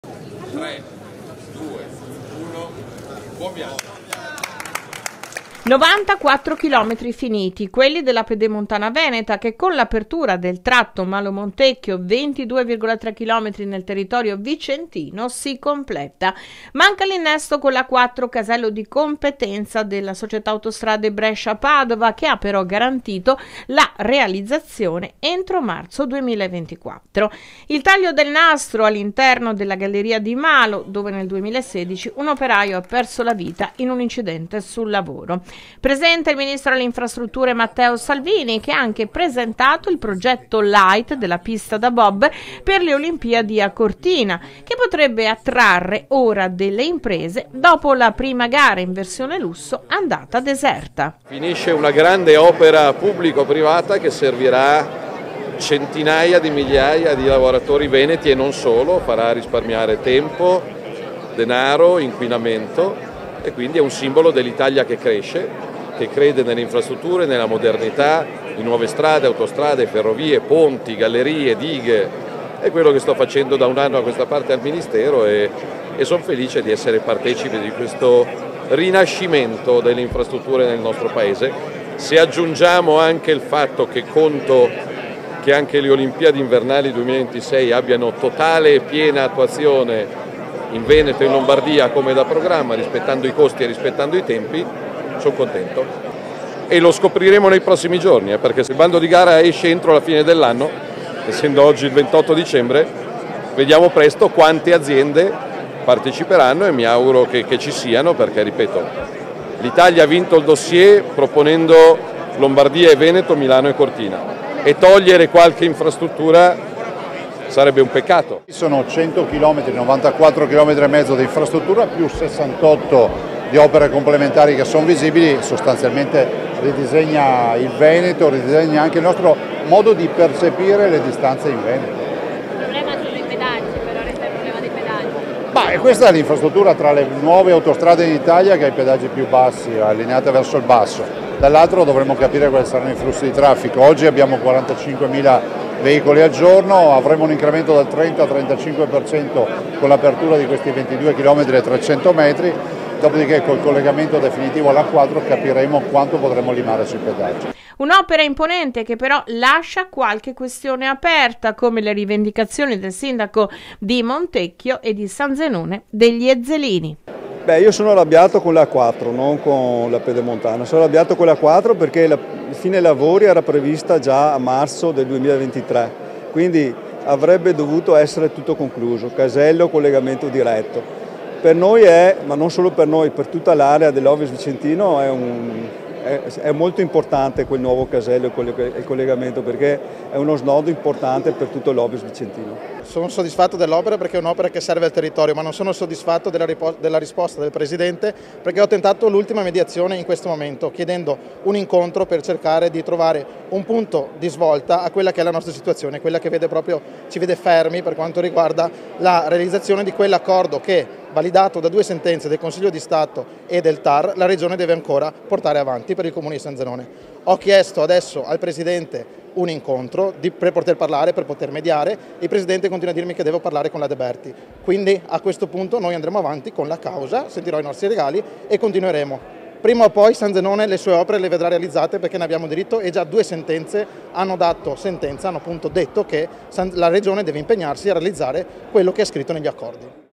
Pedemontana ultimata
Malo (Vi):  Manca solo il casello con l’innesto in A4. Domani alle 18 apre a Malo la Pedemontana ultimo tratto della superstrada. Il ministro Salvini si è detto fiducioso che le imprese siano allettate dal nuovo progetto per la pista di bob di Cortina d’Ampezzo.